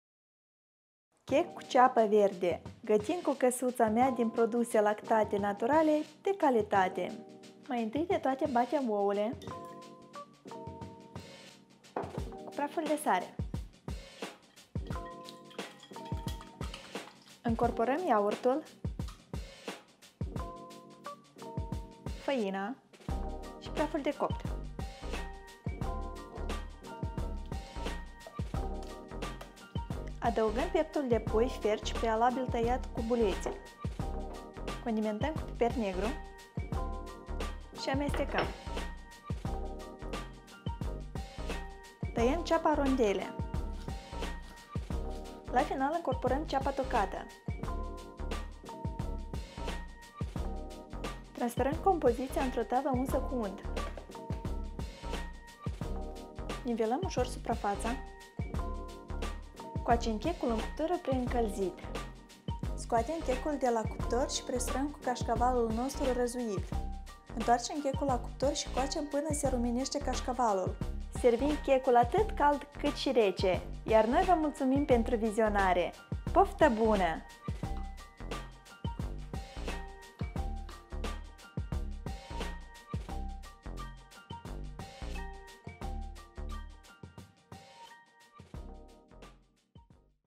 Chec cu ceapă verde
🤗 Și pentru că ziua de duminică e pentru a petrece timpul plăcut în compania prietenilor și familiei, gătiți această rețetă și împărțiți bucuria de a sta la masă cu ei în timp ce vă povestiți.

Timp: 50 minute
Porții: 8

Ingrediente:
▪️ 150 gr- piept de pui
▪️ 3 ouă
▪️ 150 gr- iaurt grecesc
▪️ 200 gr- ceapă verde
▪️ 1 lingură- făina
▪️ 70 gr- cașcaval
▪️ 1 linguriță- praf de copt
▪️ sare & piper negru

Metodă:
1. Batem ouăle cu sarea, adăugăm iaurtul, făina & praful de copt,  amestecăm până numai avem cocoloașe.

2. Adăugăm pieptul de pui prealabil fiert, condimentăm cu piper negru.

3. Tocăm ceapa verde rondele & adăugăm peste restul ingredientelor .
Amestecăm totul bine.
Transferăm într-o formă cu Diametru de 26 cm.

4. Coacem la Temperatura 180 C- 25 minute.
Scoatem de la cuptor presurăm cu cașcaval răzuit & întoarcem la cuptor încă pentru 15-20 de minute sau  până se rumenește cașcavalul.

5. Scoatem , lăsăm puțin să se răcorească & servim atât cald cât & rece.
Poftă Buna!